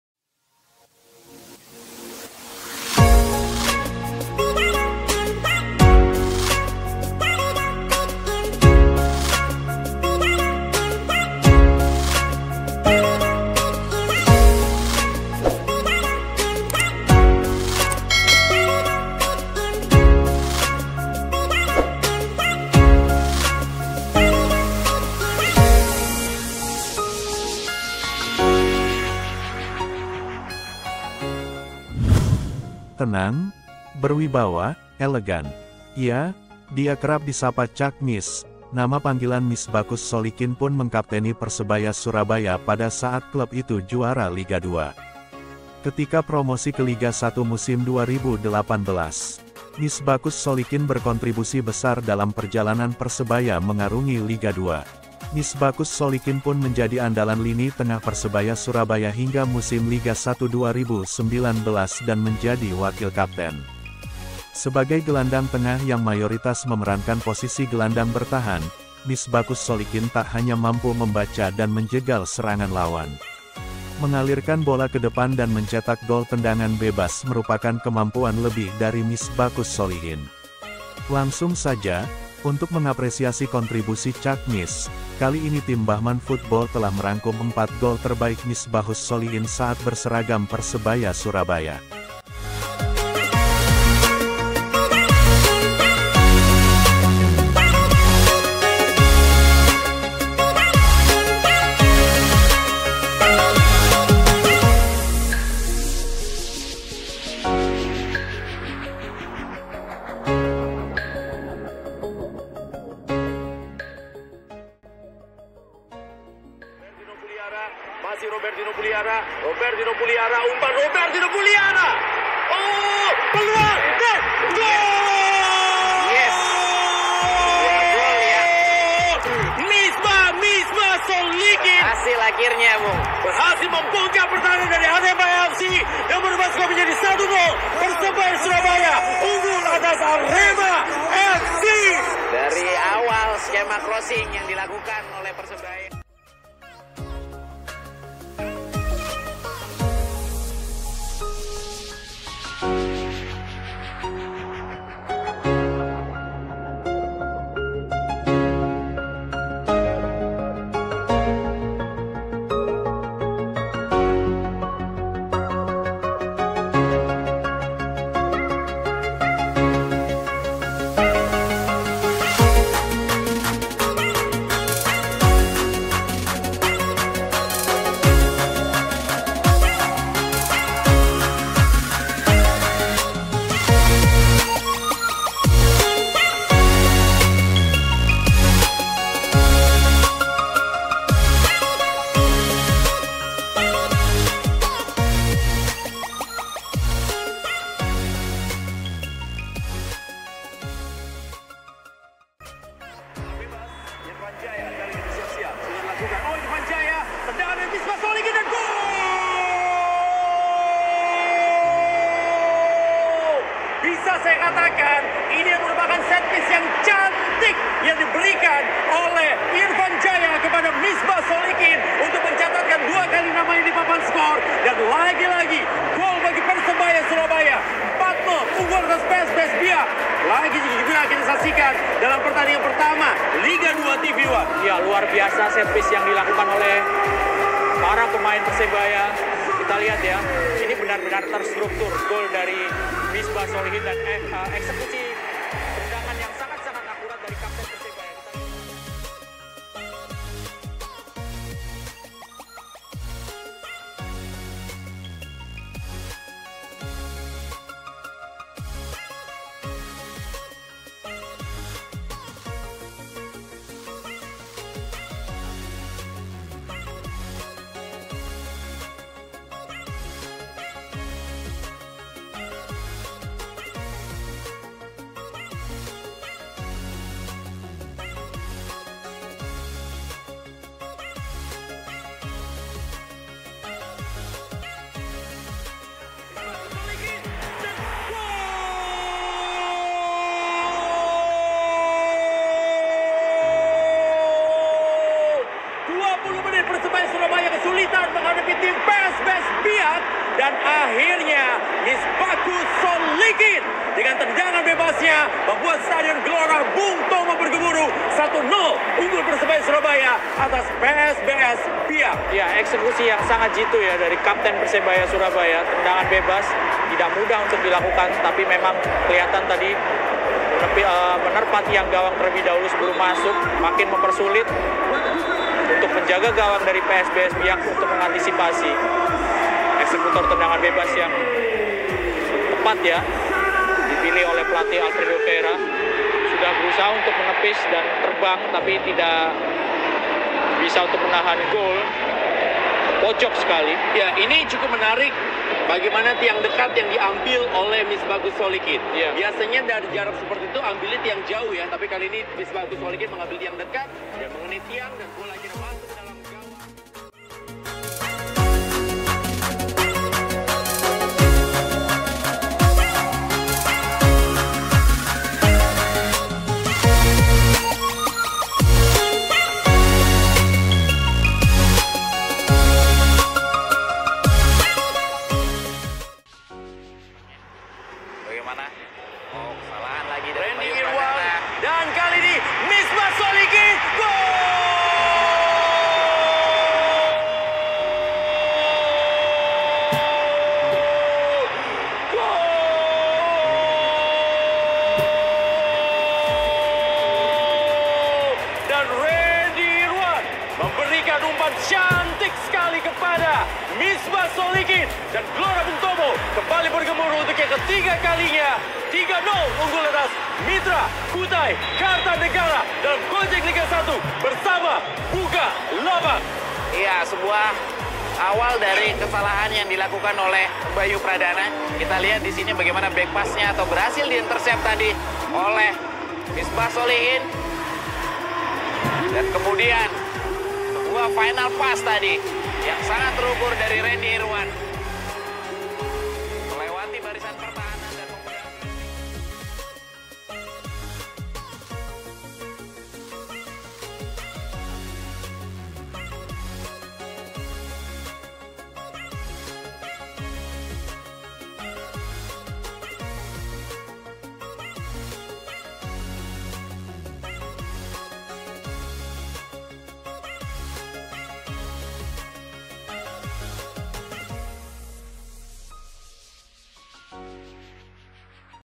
"TAMPAK SANTAI TAPI JENIUS" Inilah Deretan Gol Spesial Dari Misbakhus Sholihin Yang Selalu Dikenang
inilah 4 gol terbaik dari mantan pemain Persebaya yaitu misbahus Solihin selama berkarir dan berseragam Persebaya dari liga 2 hingga Liga 1 tahun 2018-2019